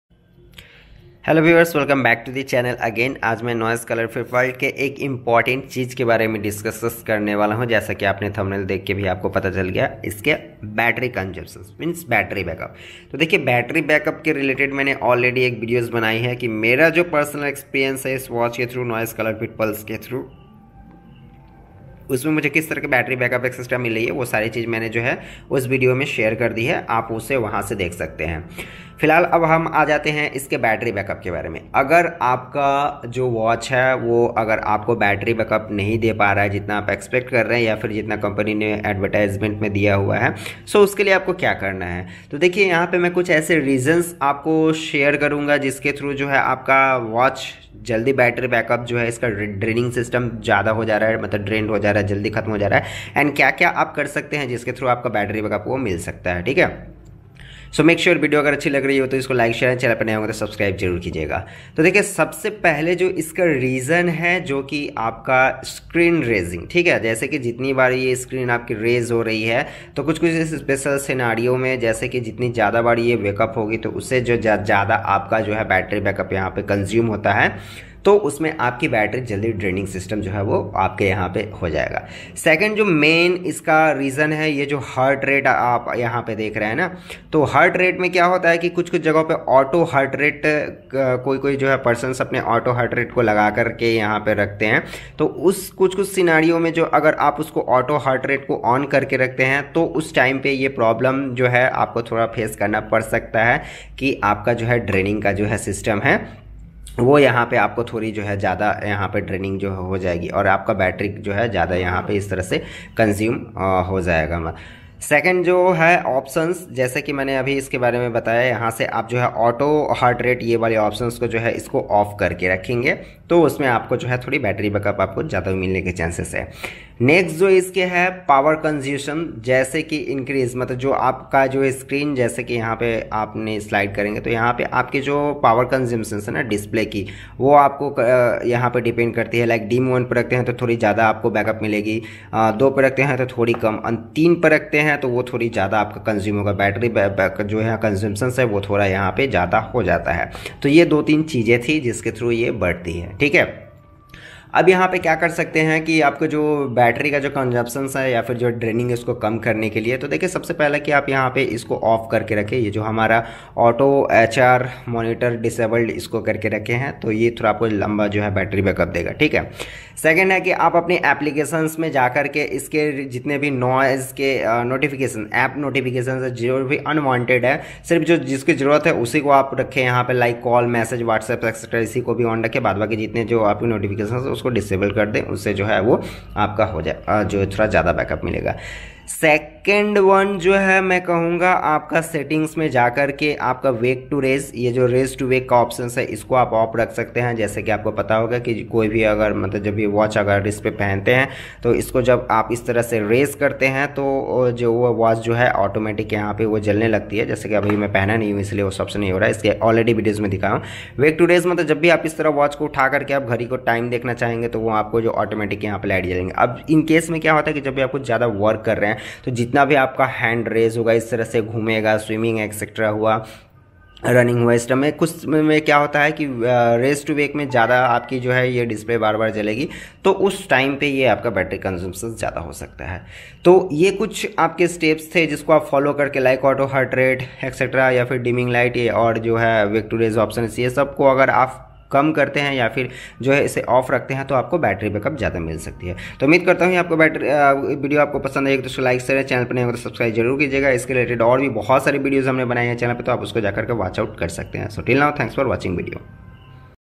Noise Colorfit Pulse Battery Draining To Fast Problem || How Get Long Battery Life For Noise Watch
In this video - Noise Colorfit Pulse Battery Draining To Fast Problem || How Get Long Battery Life For Noise Watch